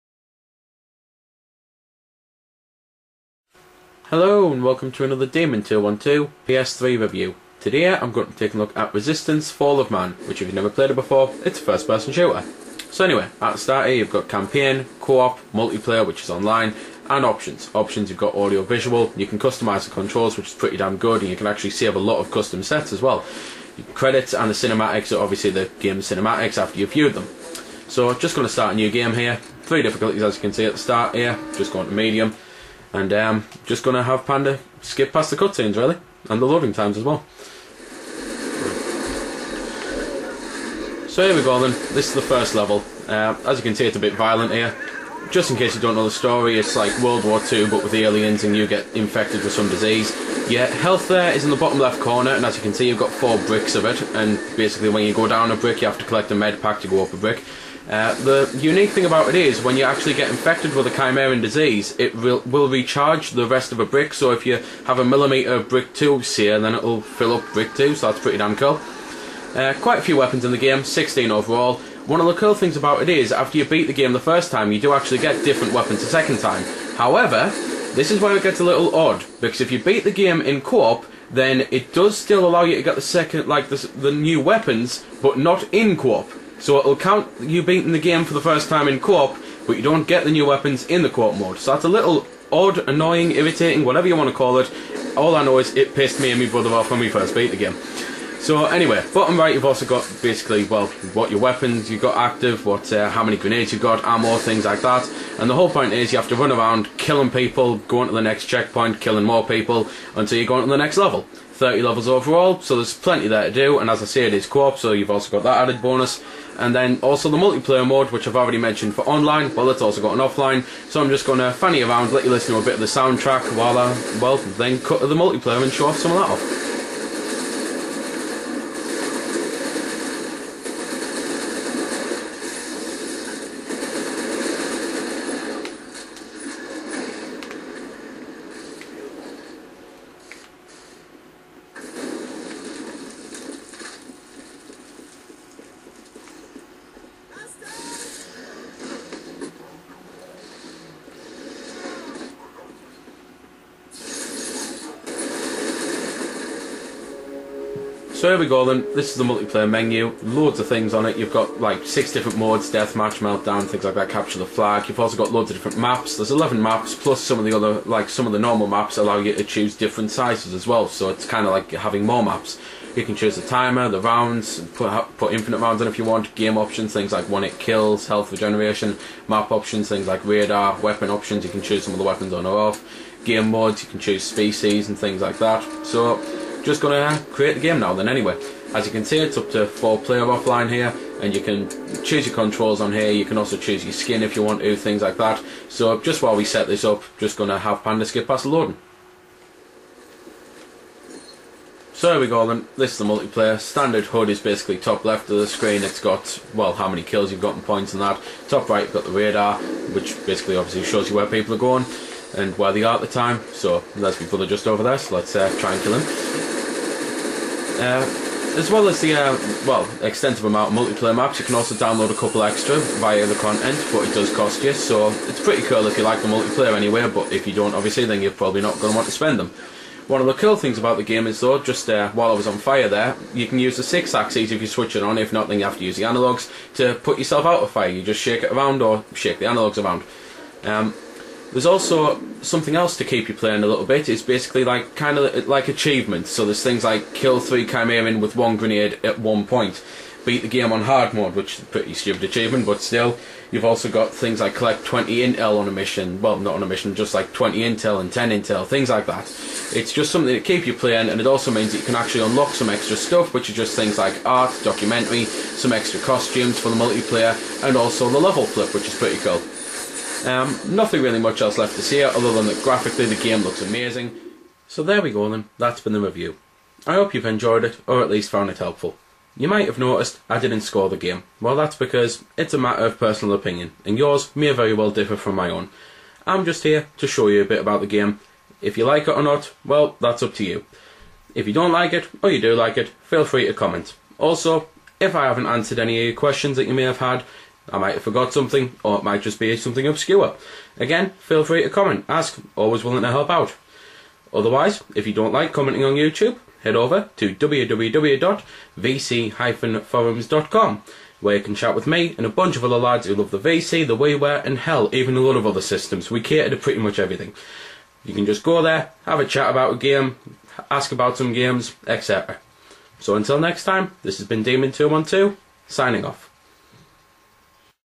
Resistance: Fall Of Man PS3 Review (MATURE VIDEO CONTENT WARNING!)
This is a review of the PS3 game Resistance: Fall Of Man.